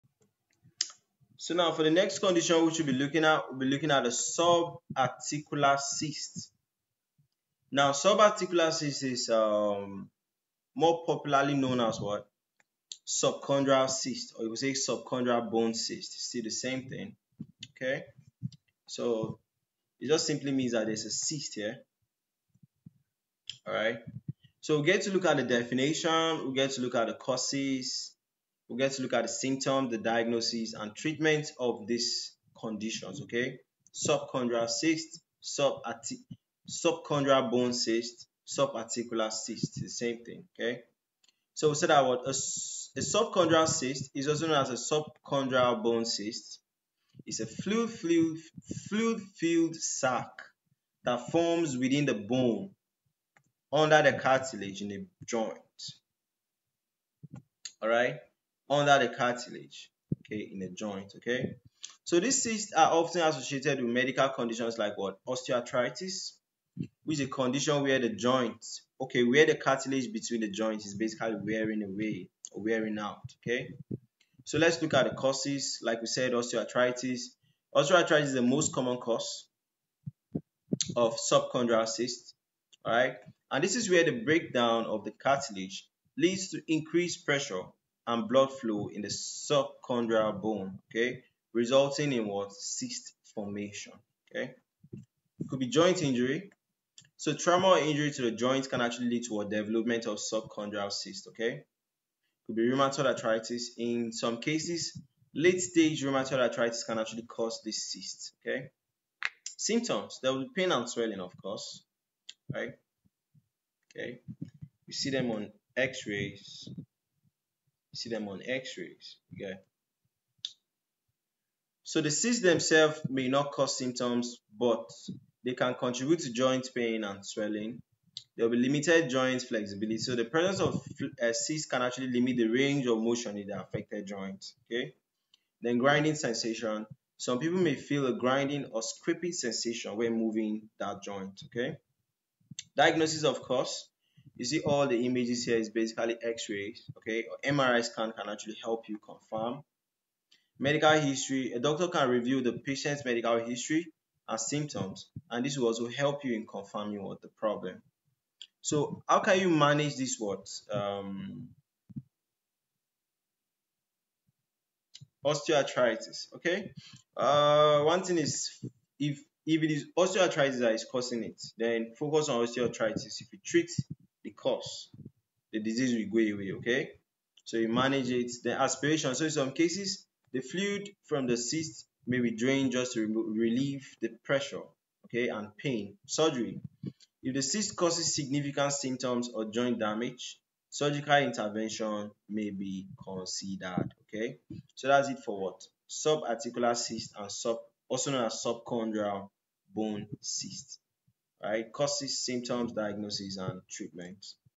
Subarticular Cyst or Subchondral Bone Cyst ; X-rays, Causes, Symptoms, Diagnosis, Treatment
A subchondral cyst, also known as a subchondral bone cyst, is a fluid-filled sac that forms in the subchondral bone, which is the layer of bone just under the cartilage in a joint. These cysts are often associated with osteoarthritis and can cause pain and swelling in the affected joint.

Causes
Osteoarthritis: The most common cause, where the breakdown of cartilage leads to increased friction and the formation of cysts.

Joint Injuries: Trauma or injury to a joint can also lead to the development of subchondral cysts.

Rheumatoid Arthritis: In some cases, late-stage rheumatoid arthritis can cause cysts to form.

Symptoms
Pain: Often felt in the joint, especially during movement.

Swelling: The joint may become swollen due to the presence of the cyst.

Stiffness: Reduced range of motion and stiffness in the joint.

Weakness: Over time, the cyst can lead to muscle weakness and difficulty performing daily activities.

Diagnosis
Imaging: X-rays or MRI scans are used to detect subchondral cysts and assess their size and location.

Treatment
Conservative Treatments: Pain management with over-the-counter pain medications, physical therapy, and lifestyle modifications.

Drainage: In some cases, a needle may be used to drain the cyst if it causes significant symptoms.

Surgical Intervention: Surgery may be considered if conservative treatments are ineffective and the cyst causes severe symptoms.

Would you like more detailed information on any specific aspect of subchondral cysts?